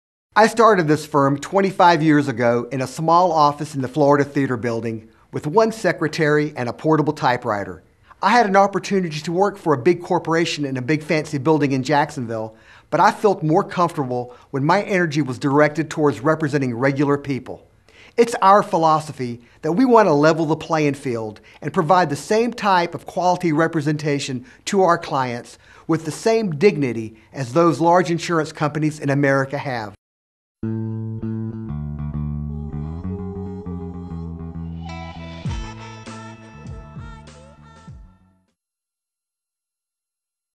Farah & Farah
Farah & Farah fights for ordinary people in Jacksonville, FL.  He helps clients receive fair compensation for their injuries in civil suits.  For over 25 years, Eddie Farah has been fighting big companies for everyday people.